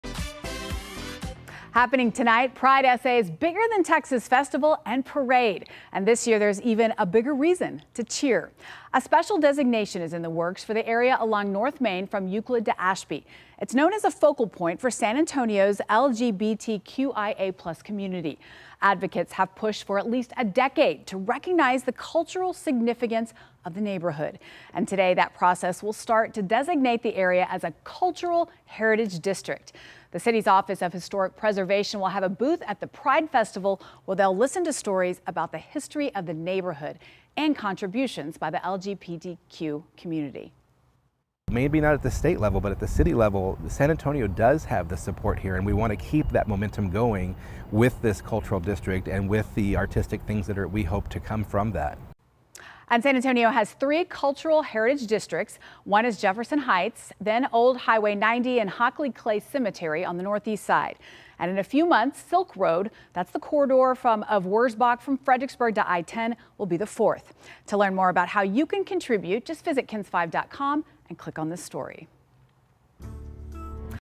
Special destination in works for LGBTQIA+ community in San Antonio
The process to designate the area as a "Cultural Heritage District" is underway.